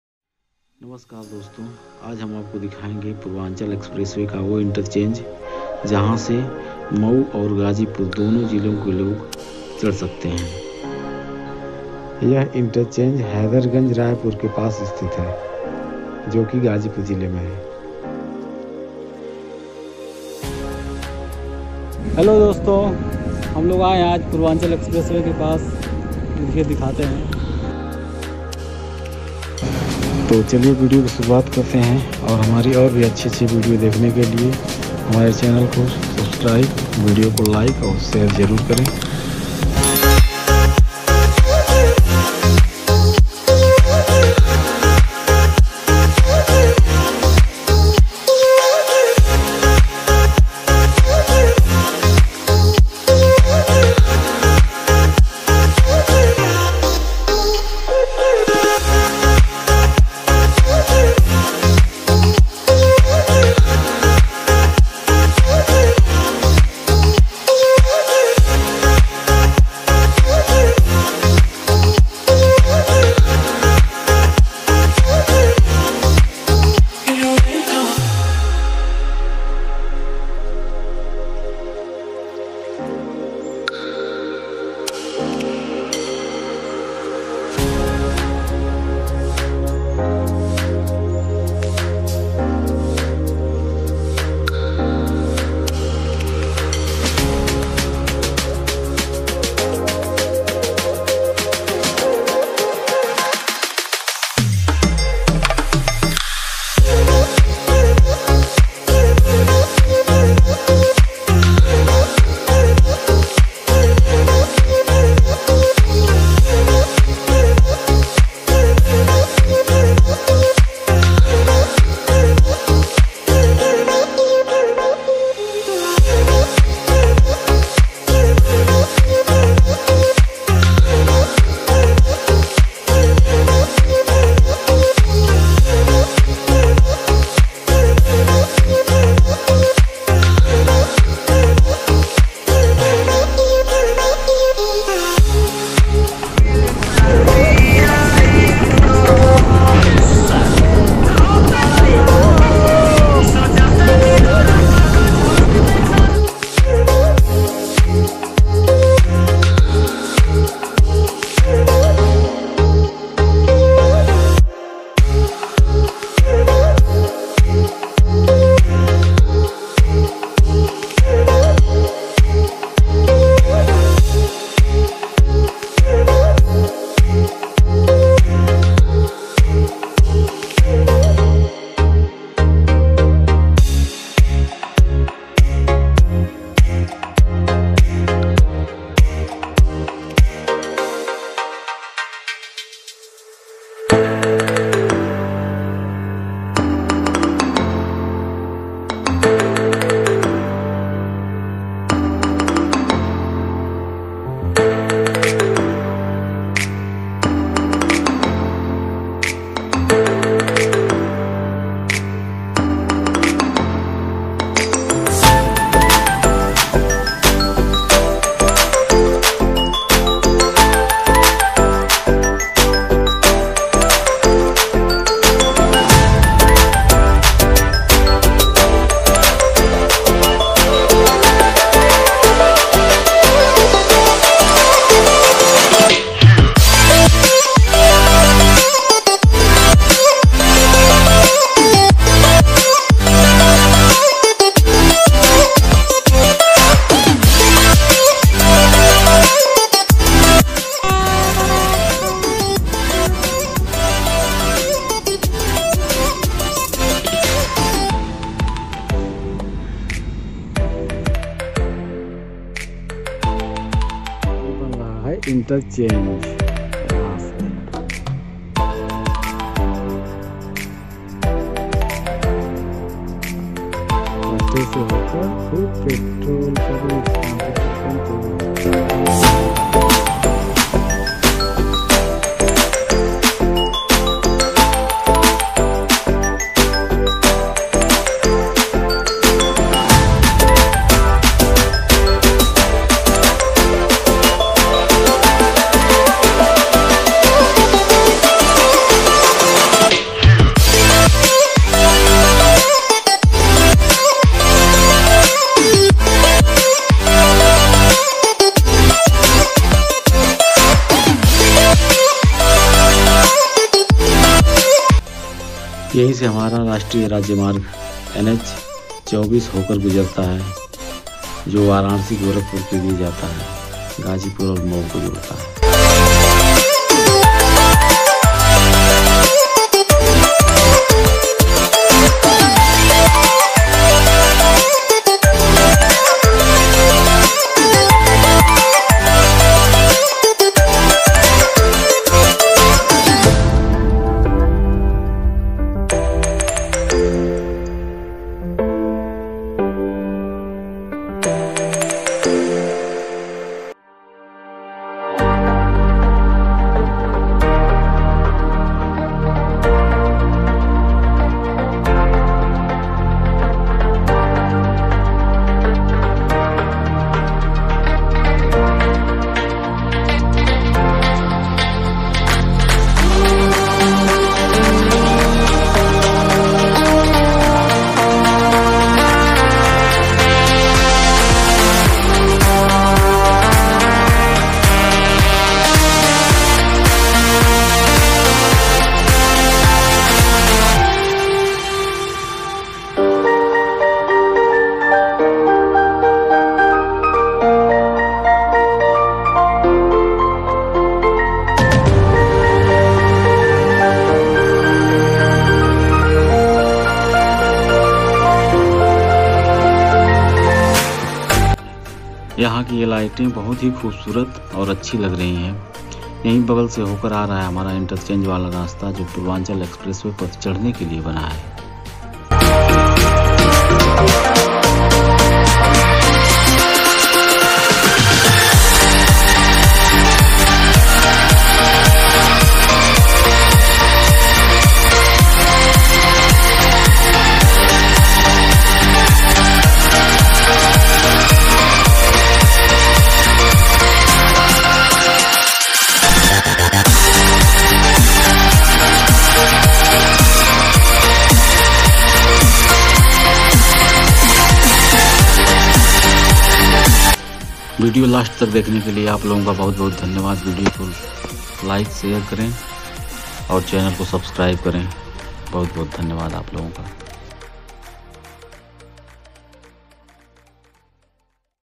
Purvanchal Expressway Interchange
आज हम आपको दिखाएंगे पूर्वांचल एक्सप्रेसवे का वो इंटरचेंज जहां से मऊ और गाजीपुर दोनो जिलों के लोग पूर्वांचल एक्सप्रेसवे का आनंद ले सकेंगे।